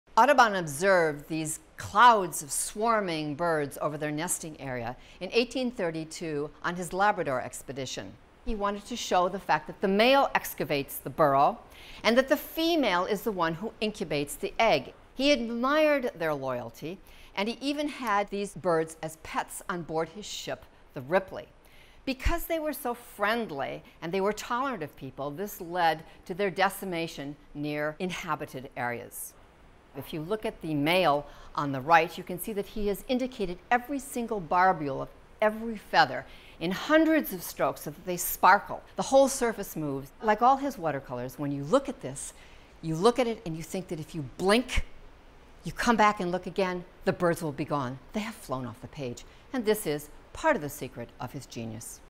Atlantic Puffin by John James Audubon
Curator Roberta Olson discusses John James Audubon's watercolor study of the Atlantic Puffin for The Birds of America.

Audubon's Aviary: Parts Unknown (on exhibit March 21-May 26, 2014) will consider John James Audubon as an established artist-naturalist, a world citizen, and a celebrity in an expanding nation. Most of the watercolors in Parts Unknown (studies for Havell plates 176-305) depict water birds, which are among Audubon's most spectacular and largest birds, with numerous studies begun during his southeastern explorations and on his Labrador Expedition.